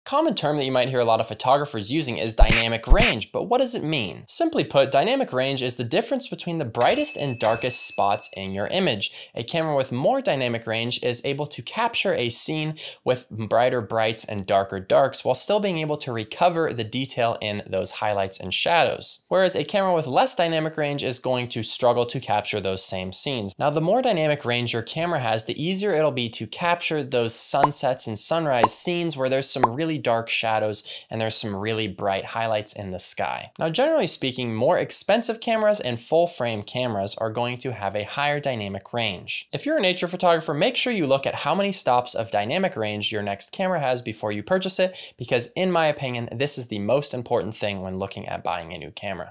Dynamic Range in Photography Explained
Let’s talk about dynamic range… ⬇️

It’s what separates your smart phone camera from a true DSLR or Mirrorless camera. Dynamic range is the difference between the darkest spot in your photo and the brightest spot in your photo.

As landscape photographers, dynamic range is SUPER important. Since you’ll want to capture great sunsets and sunrises, you’ll be demanding the most out of your camera to capture the bright sun in contrast with the dark foreground. Next time you are buying a camera, look for one with the most dynamic range possible!